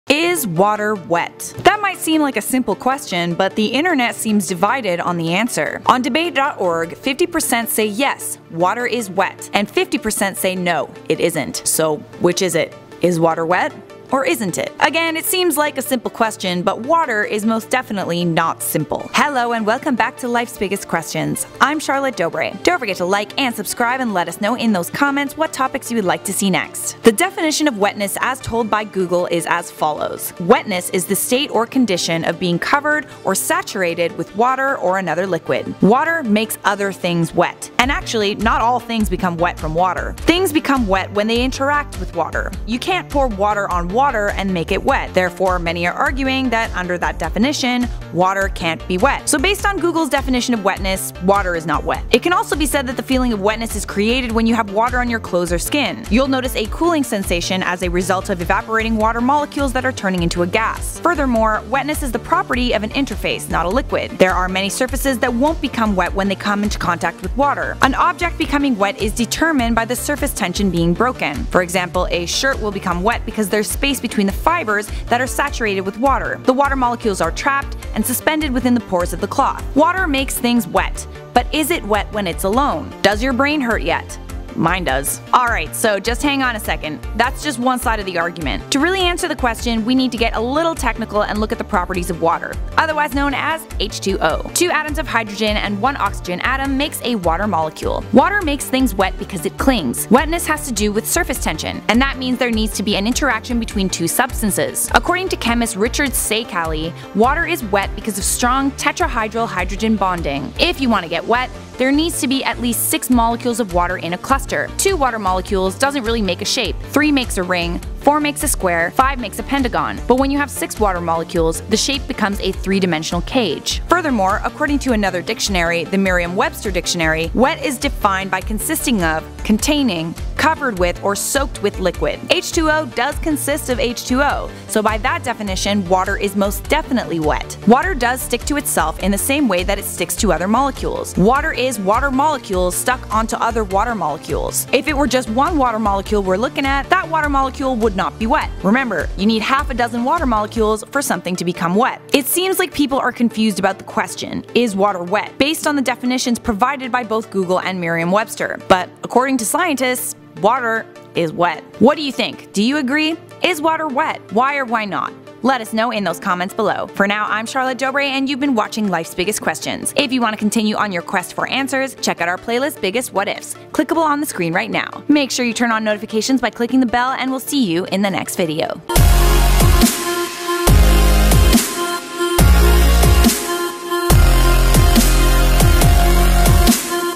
Is Water Wet?
Is water wet? It might seem like a simple question, but The internet seems divided. on debate.org, 50 percent say yes water is wet, and 50 percent say no it isn’t.  So which is it? is water wet? Or isn’t it? Again, it seems like a simple question, but water is most definitely not simple.